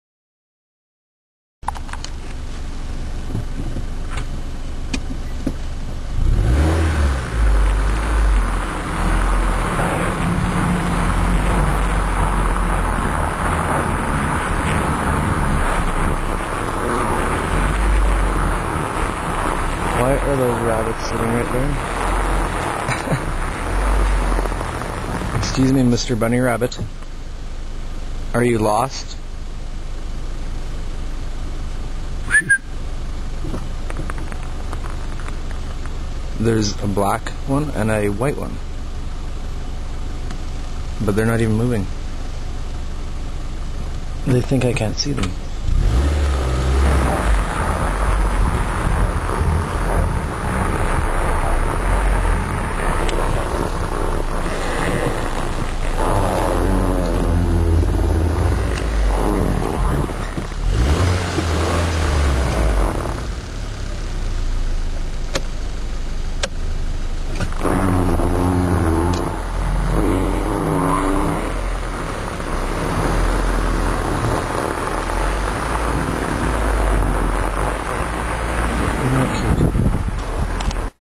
Snow Bunnies
Here are some cute bunnies (rabbits) lost on the snowy streets of Saskatoon.